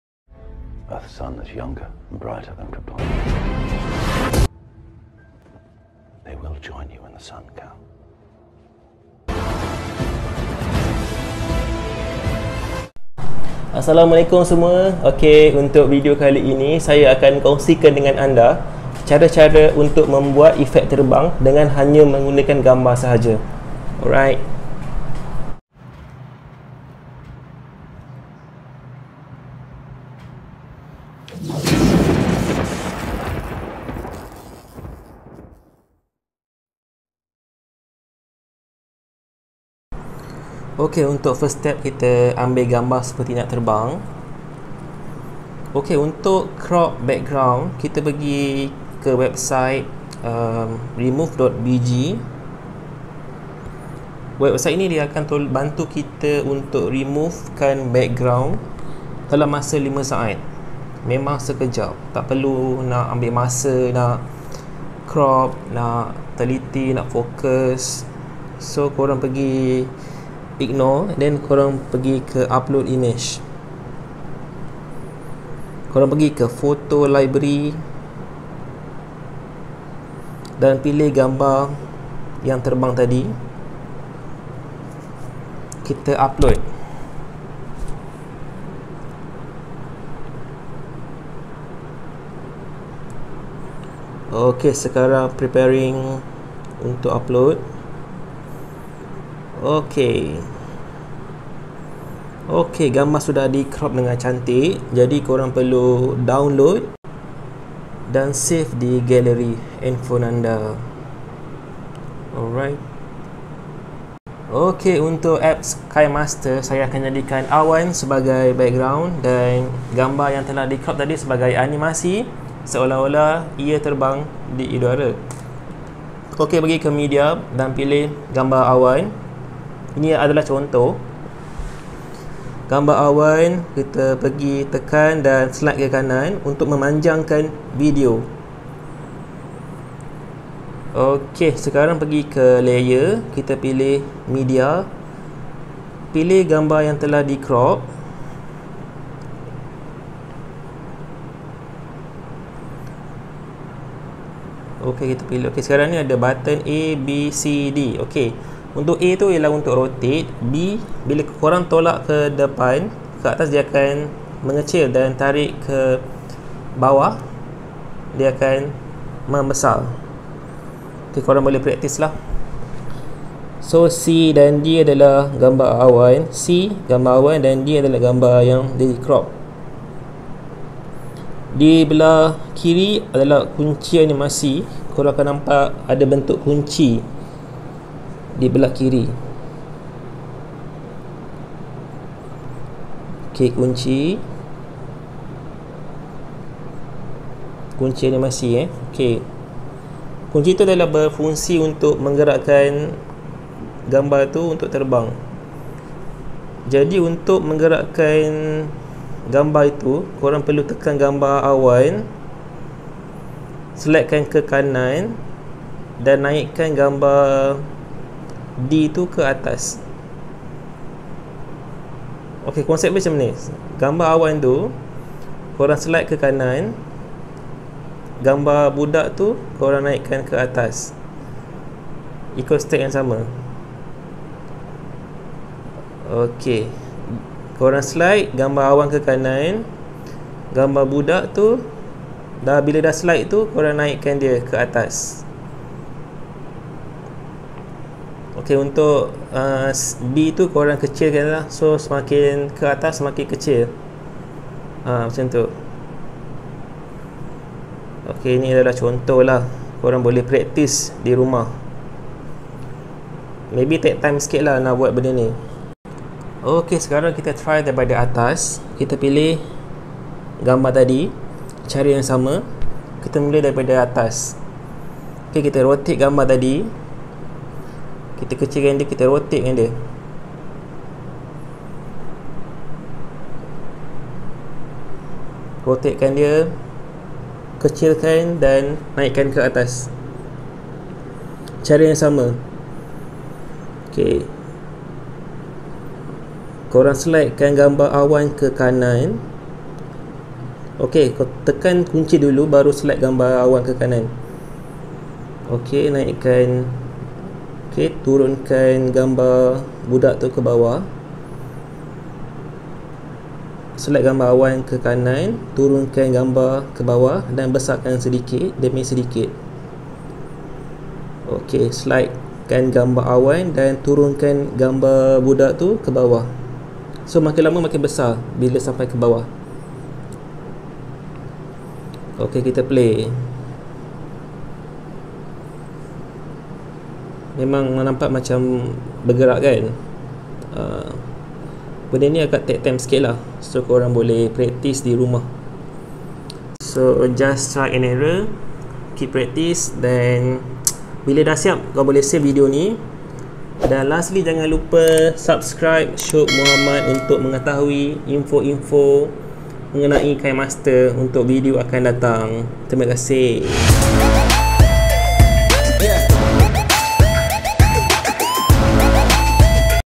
CARA BUAT VIDEO TERBANG PALING MUDAH
Hi, Assalamualaikum dan Salam sejahtera,
Dalam video ini saya kongsikan cara nak edit video Terbang dengan menggunakan gambar.

Jika anda rasa video ini bermanfaat, kongsikan video ini kepada kawan-kawan anda.

Banyak lagi video editing yang akan menyusul